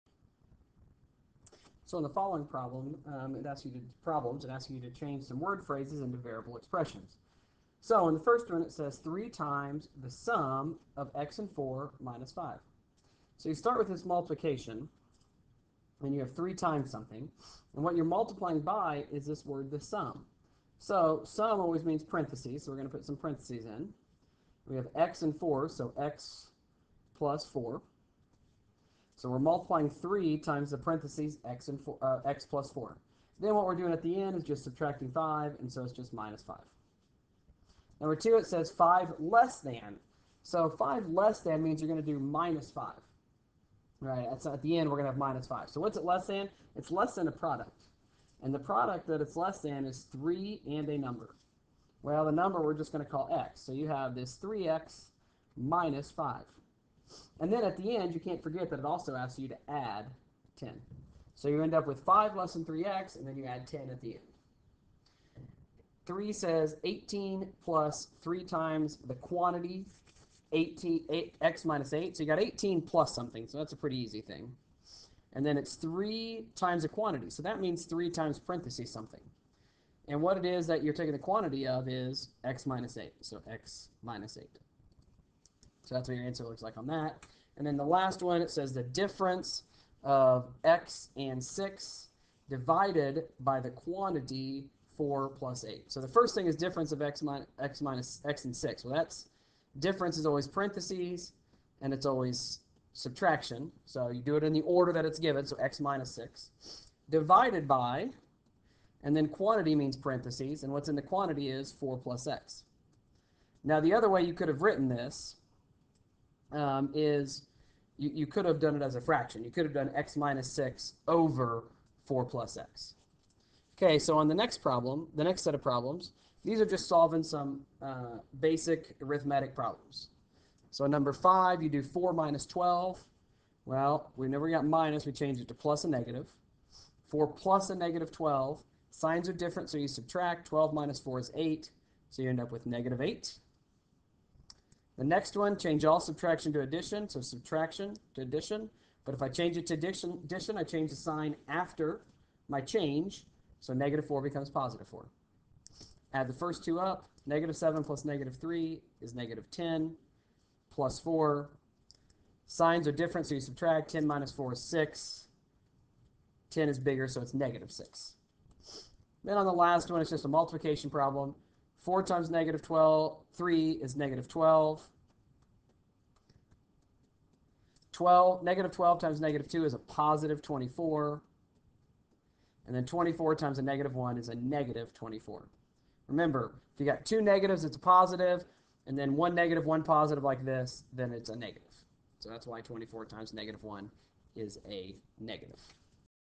Verbal Expressions to Variable Expressions
Some examples on taking a sentence and turning it into a variable expression.